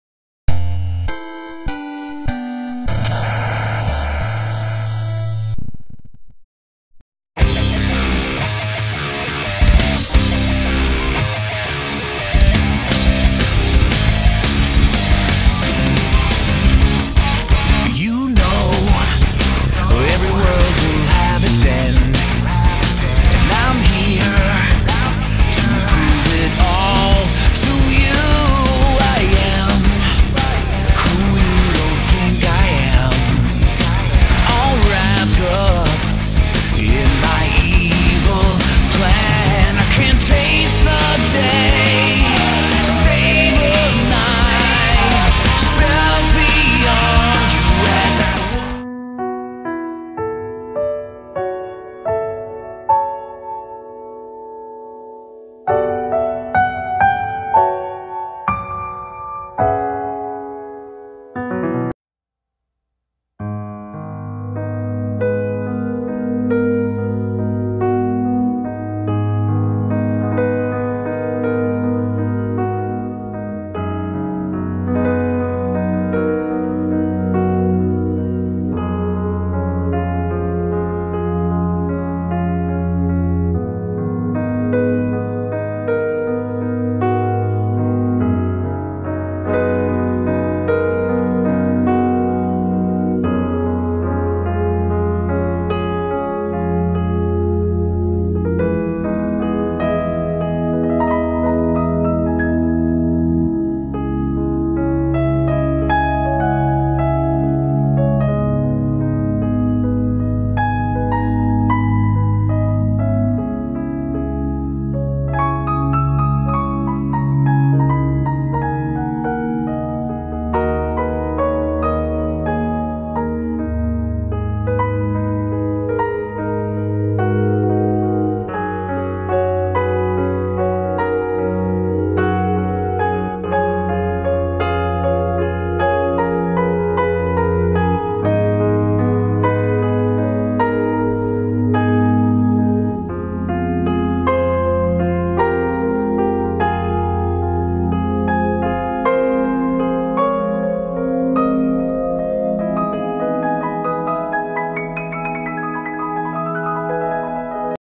Sonic /Sonamy - True love never go away [comic]
takie tam coś na szybko :P bez napisów, bez głosów, sama historyjka obrazkowa. Chyba nie trzeba tekstów, by zrozumieć sens :P